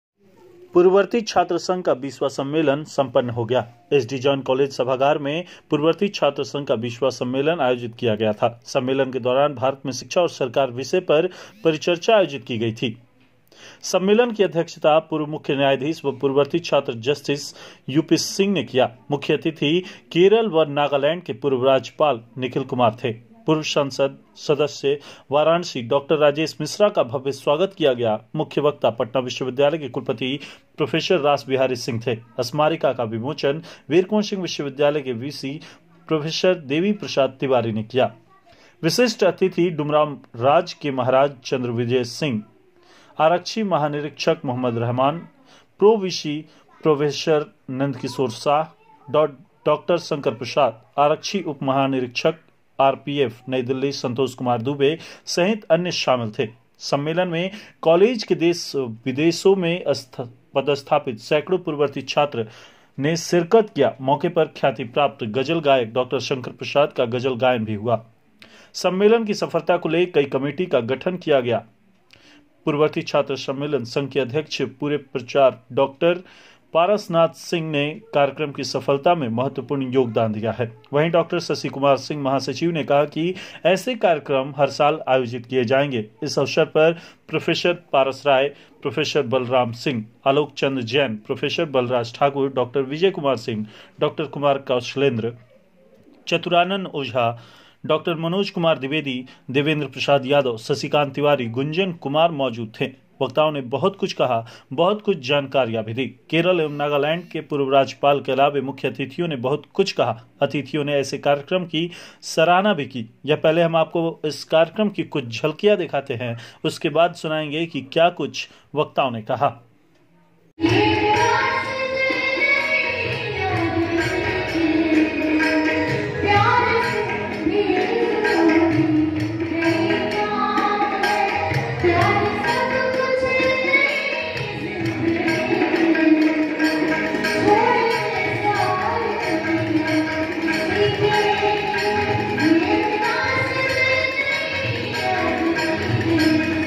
आरा में पूर्ववर्ती_छात्र_संघ का 20वां सम्मेलन संपन्न,शिक्षा और सरकार पर परिचर्चा का आयोजन
केरल व नागालैंड के पूर्व राज्यपाल सहित अतिथि बोले- ऐसे कार्यक्रम बहुत कम स्थान पर देखने को मिलते है

जैन_कॉलेज में सम्मेलन में अतिथियों का हुआ भव्य स्वागत, शिक्षा और सरकार पर परिचर्चा